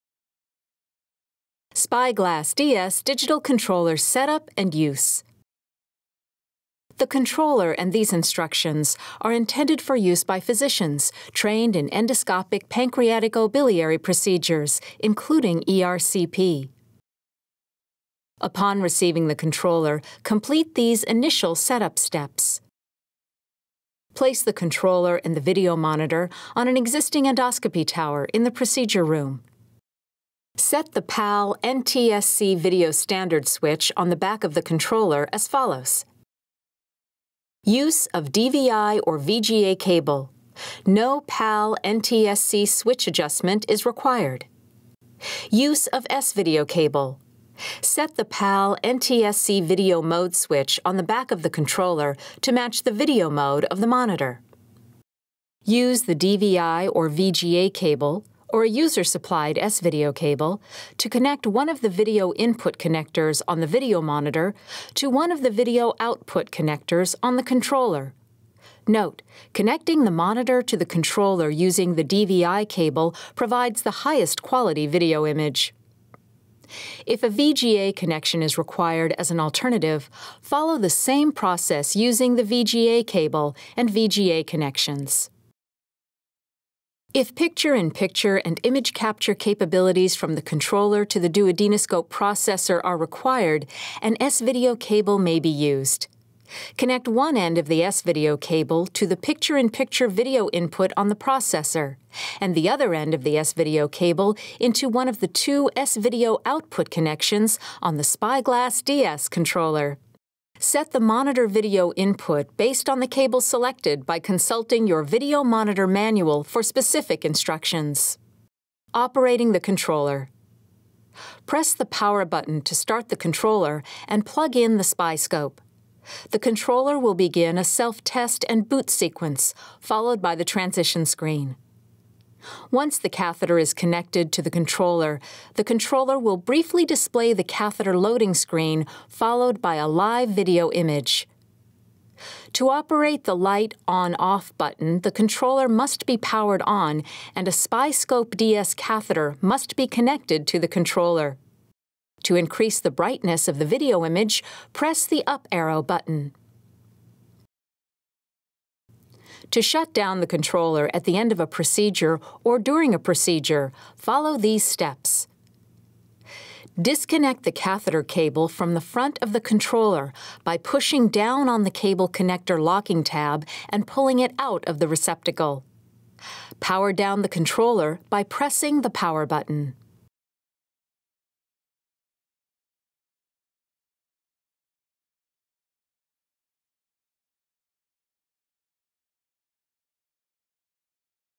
Set-up and Use of the SpyGlass™ DS System’s Digital Controller
This video tutorial is particularly useful for the initial set-up of the SpyGlass DS Digital Controller.